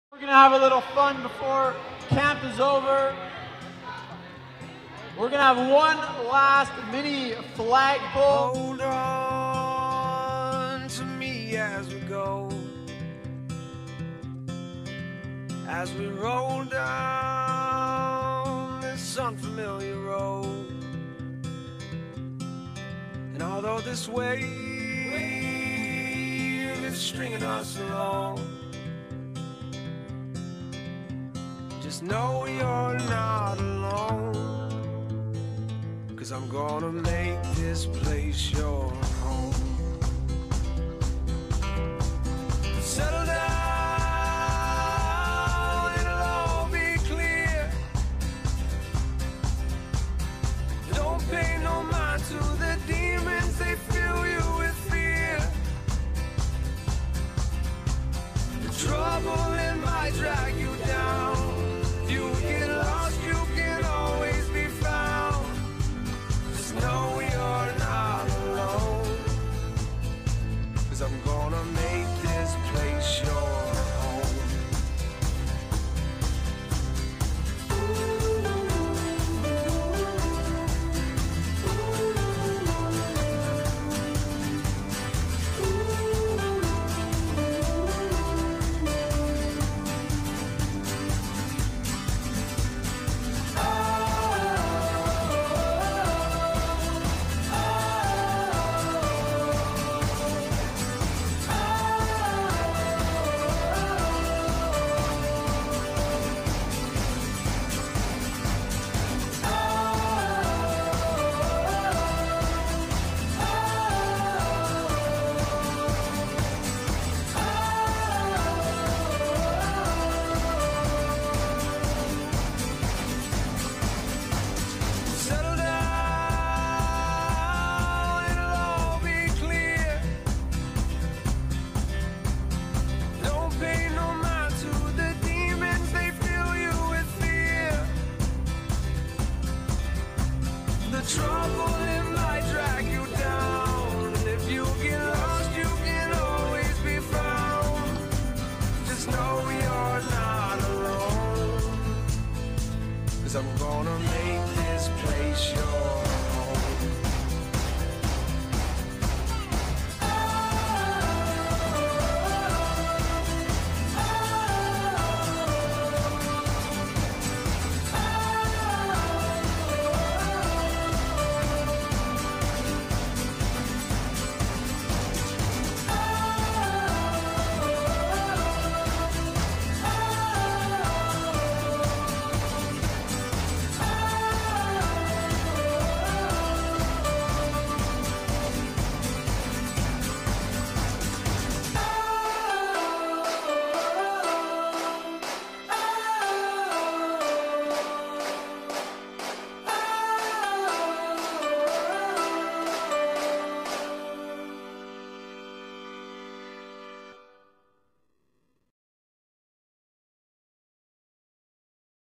Last Day of Camp - Vaughan 2013 - The Jack and Pat Kay Centre Camp
Say goodbye to summer 2013 with us as we enjoy one last flagpole together.  Looking forward to summer 2014!